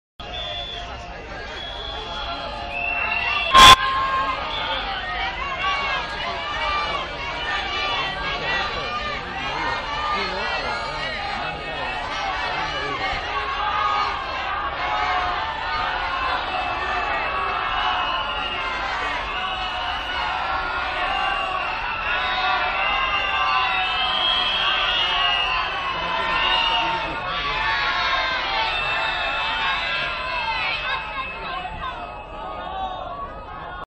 Rodrigo Rato es abucheado por los 'Afectados por la Hipoteca'
Rodrigo Rato no ha querido perderse la primera aparición pública de la secretaria general del PP tras la crisis desatada en las filas del PP por el 'caso Bárcenas'. El expresidente de Bankia ha hecho su entrada en el madrileño Hotel Ritz poco antes de las nueve de la mañana provocando la sorpresa entre los cientos de periodistas que esperaban la llegada de Rajoy. Y es que, el nombre de Rato no aparecía en la lista de invitados.